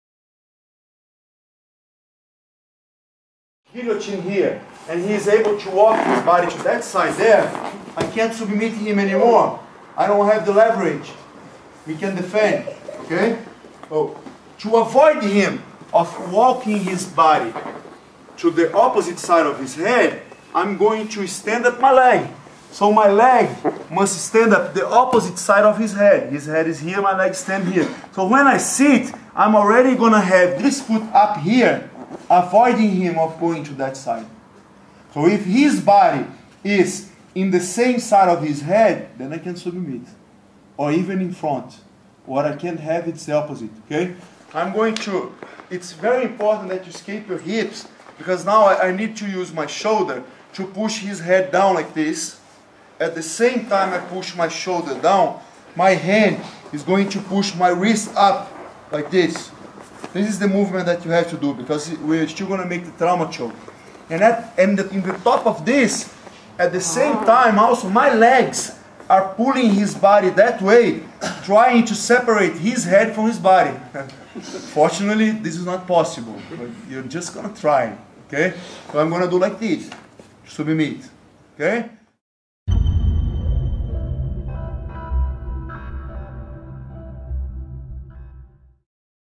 SW - Guillotine Choke
The Guillotine Choke from a Lucio Linhares seminar at Hipko 2009